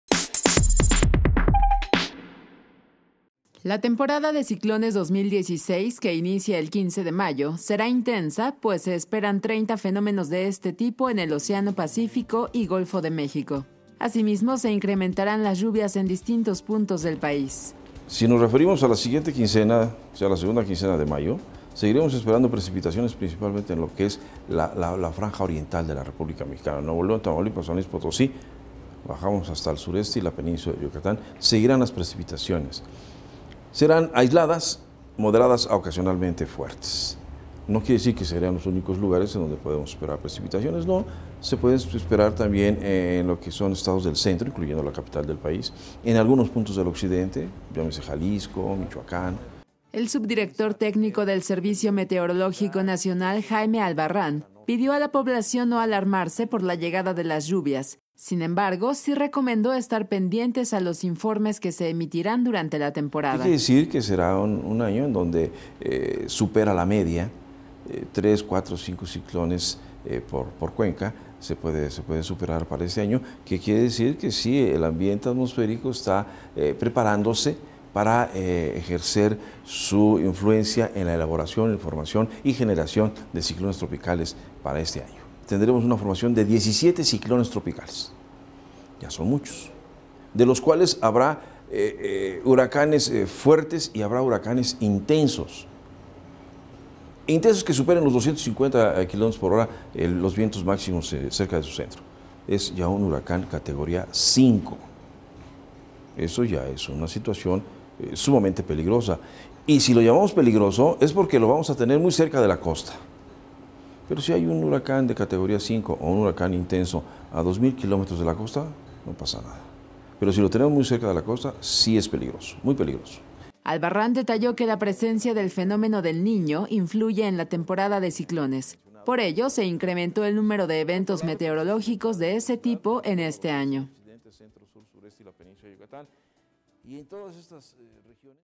Meteorológico nacional pronostica 30 ciclones para la temporada 2016
El subdirector técnino del SMN, Jaime Albarrán, señaló que se trata de 17 ciclones tropicales y 13 para el Golfo de México; pidió a la población mantenerse informada durante esta temporada